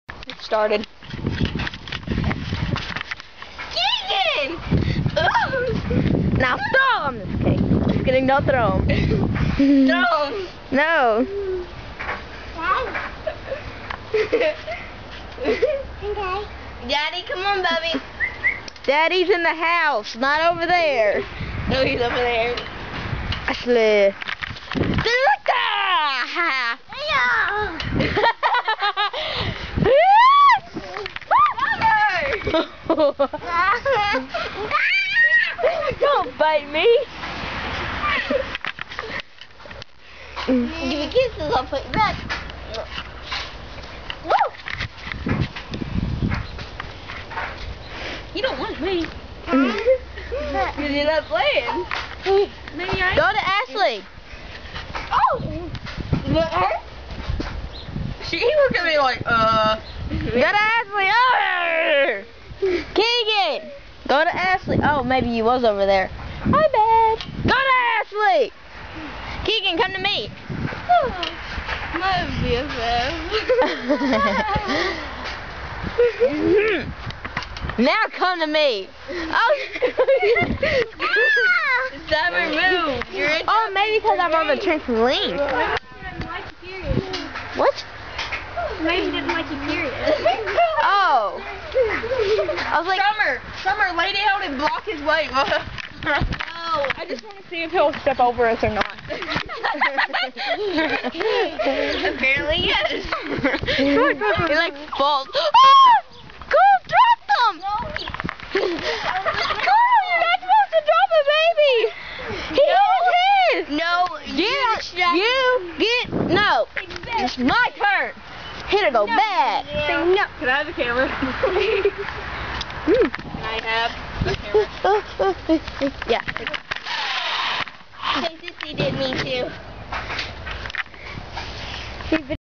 2 Year old Can't decide who he wants more
this 2 year old doesnt know who he wants to hold him...so he goes back and forth...while the camara women acts wierd. yeah.